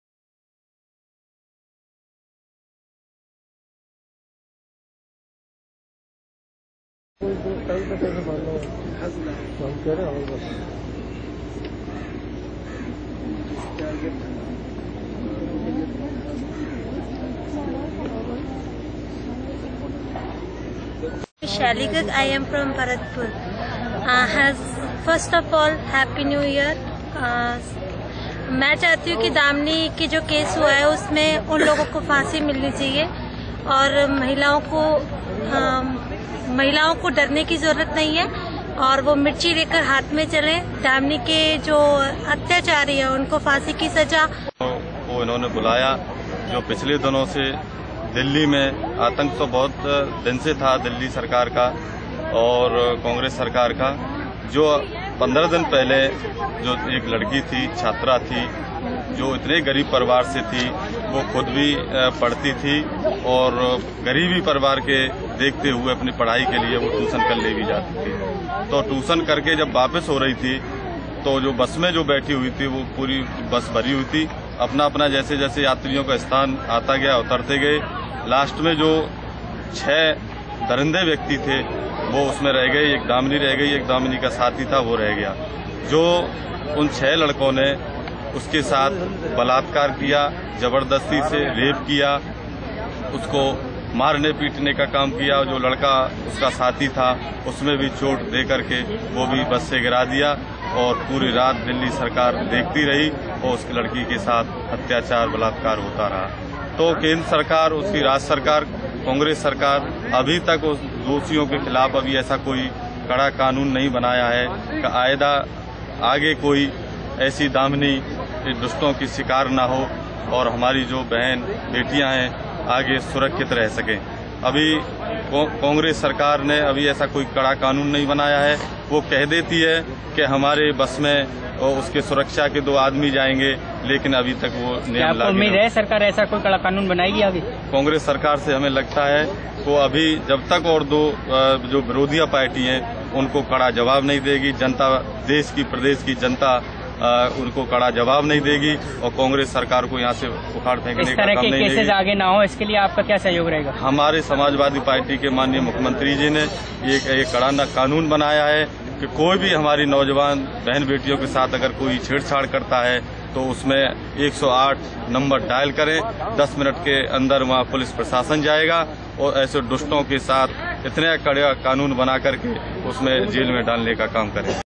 apnahindustan.com
वृन्दावन - समाजवादी पार्टी के कार्यकर्ताओं एवं स्थानीय नागरिकों ने गैंगरेप पीडिता को मोमबत्ती जलाकर भावभीनी श्रद्धांजलि अर्पित की। सभी ने एक स्वर में केन्द्र सरकार से कडा कानून बनाने की अपील की।